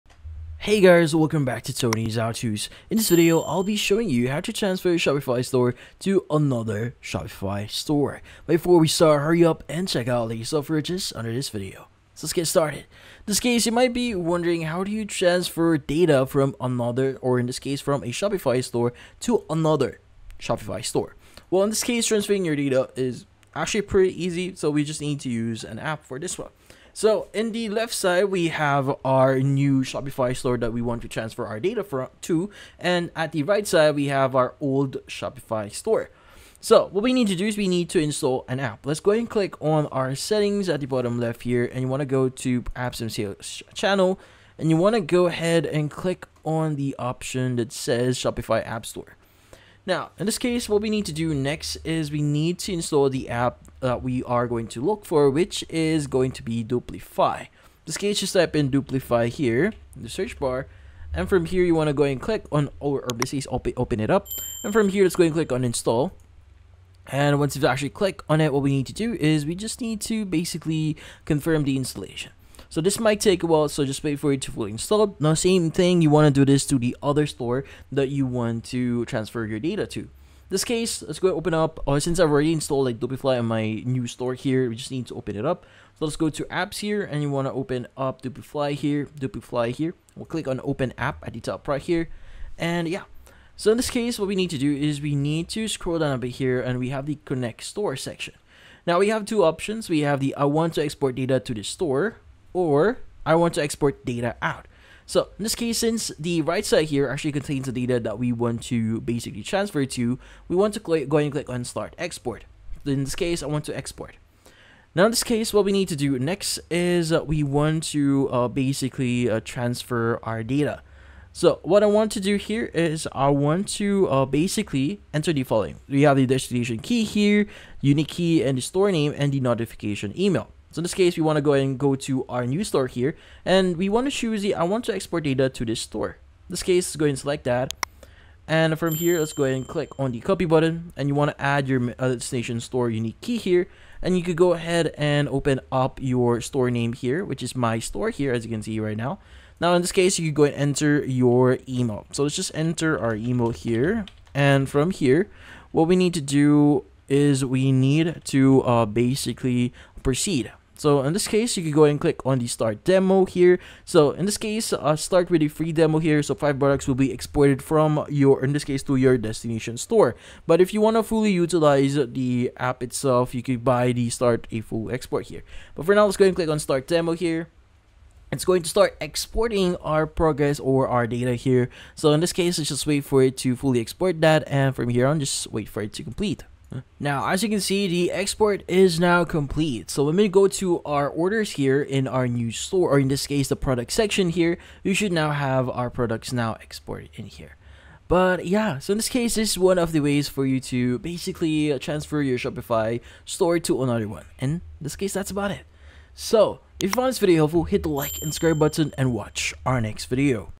How to Transfer Shopify Store to Another Shopify Store (Quick Tip, 2024)
In this video, I show you How to Transfer Shopify Store to Another Shopify Store (Quick Tip) in 2025. The quick and easy tutorial how to import products from shopify store to another shopify store. Do you want to know step by step how to copy shopify store to another shopify store in 2025? Yes? Great!